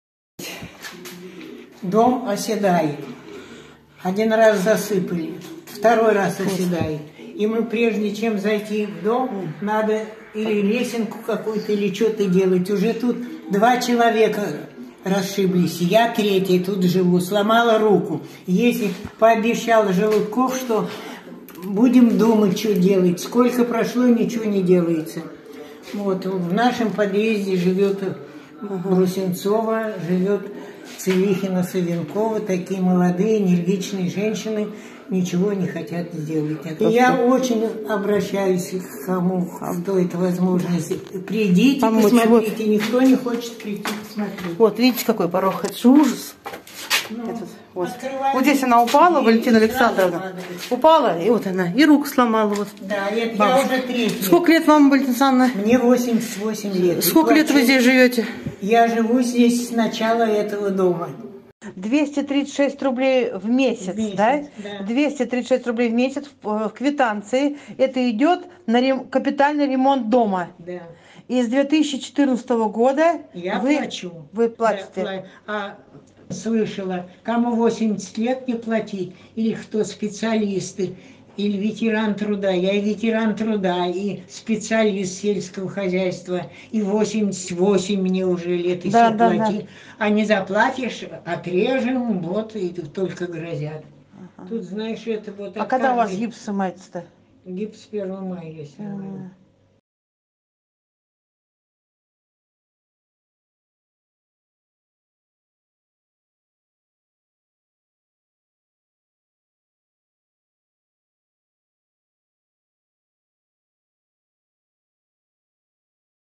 В саратовском райцентре для чиновницы сделали VIP-вход в подъезд
Жительница села Питерка Саратовской области Валентина Орлова просит местные власти отремонтировать вход в подъезд многоквартирного дома по адресу Московский переулок, 11.

Недавно 88-летняя женщина упала при входе в здание и сломала руку. По ее словам, ранее травмы получили еще два человека.

"Дом оседает. Прежде чем зайти в дом надо уже лесенку какую-то делать", - говорит пострадавшая.

Примечательно, что в том же подъезде, по словам сельчан, проживает заместитель главы района по социальной сфере Татьяна Брусенцева.

Впрочем, проблем, сходных с теми, что испытывают ее соседи, у чиновницы нет - с улицы в квартиру прорубили отдельный вход.